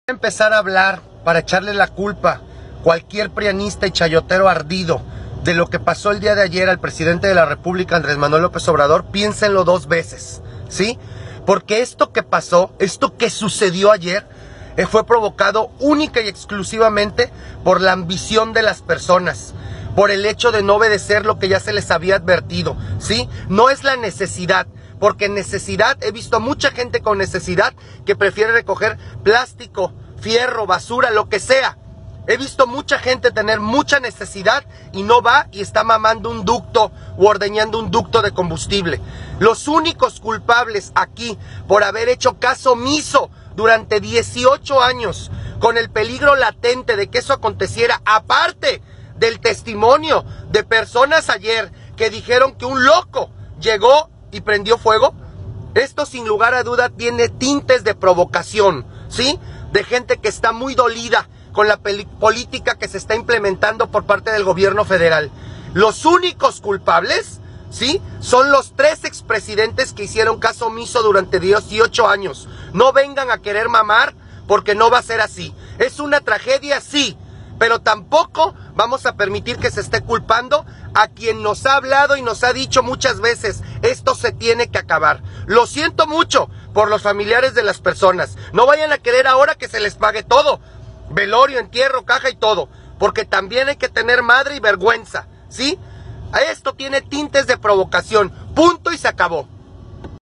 Opinión de la Explosión de los ductos de Pemex
Opinión de la Explosión de los ductos de Pemex , Los únicos Responsables son ellos y no es la Primera ves qué pasa una explosión al momento de estar Robando